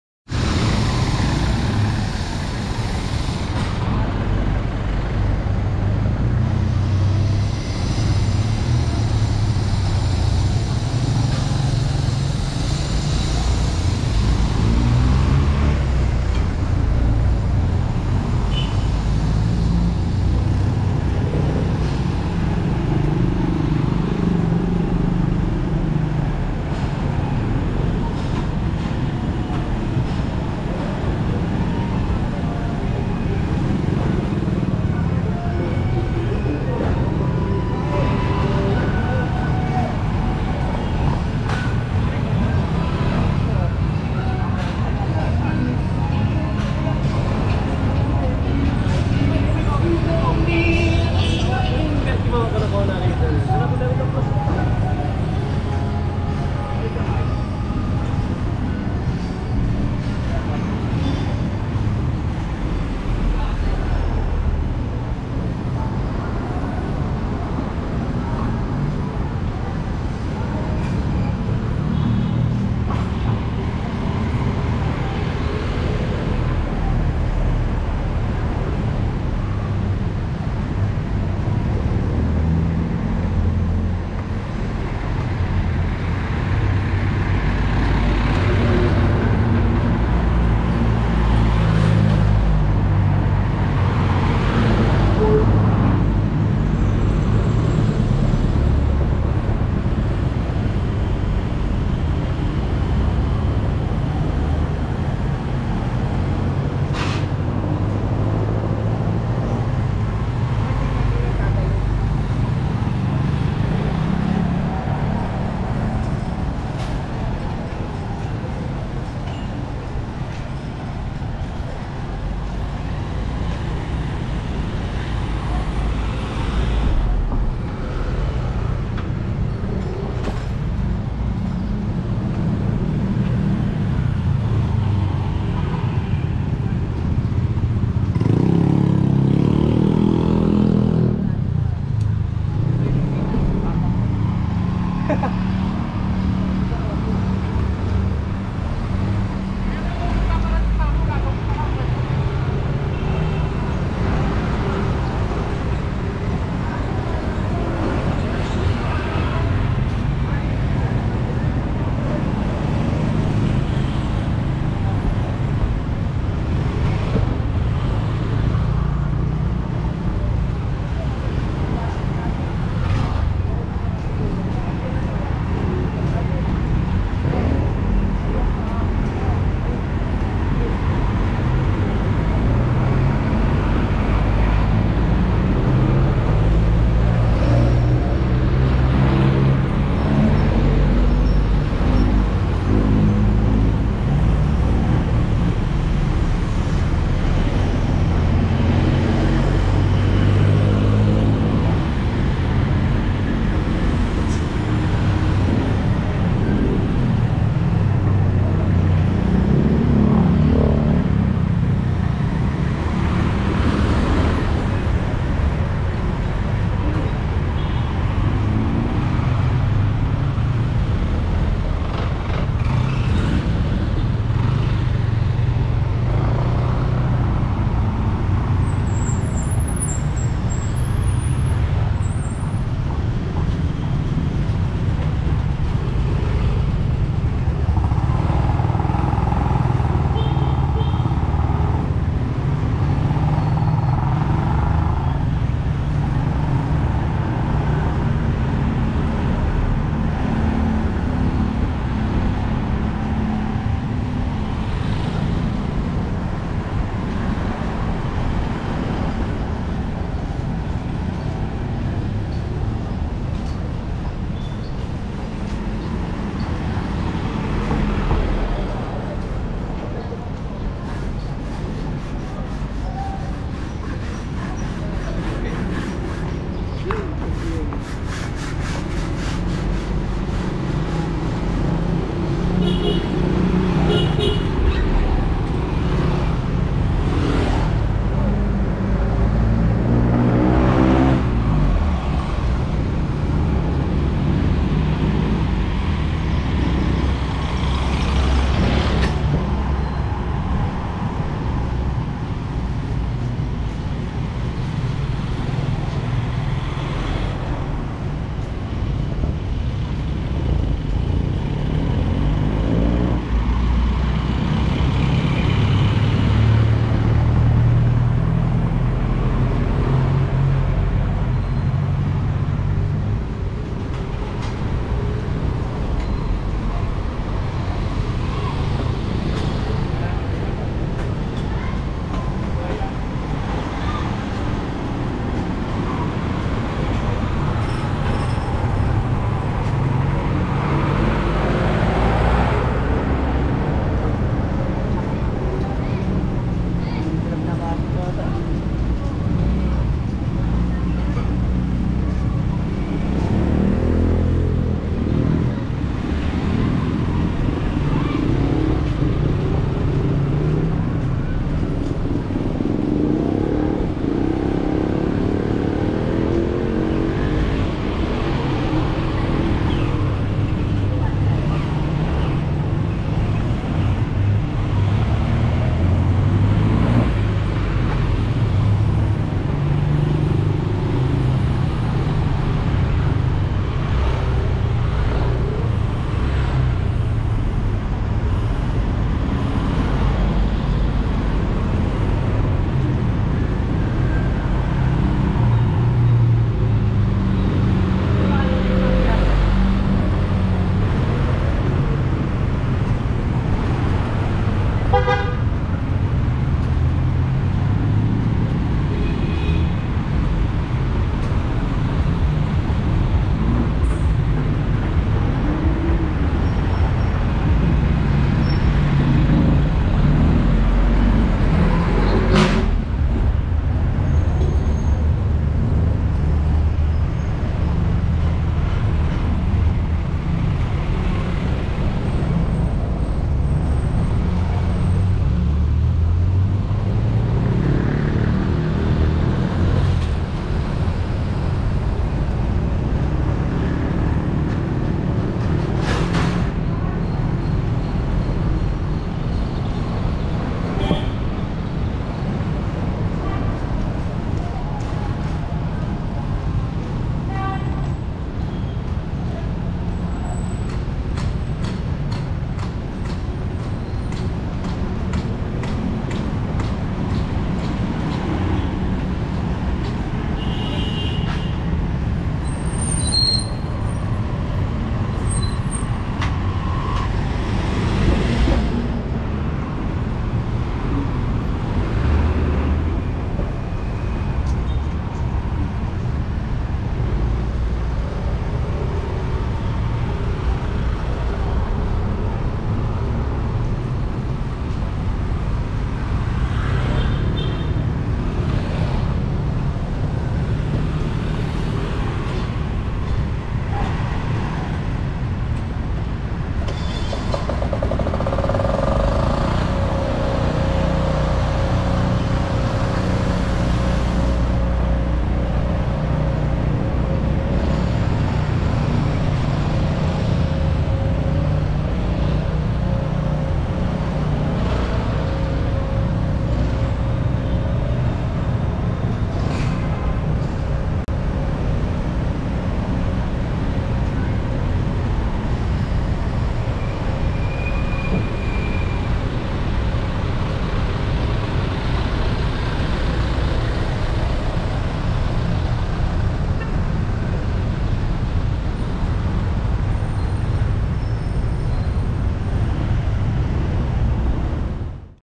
Walking Tour SH Loyola formerly Lepanto Sampaloc Manila
Sergio Loyola St., formerly Lepanto
in Sampaloc, Manila. Starting from
P. Campa to Lacson Avenue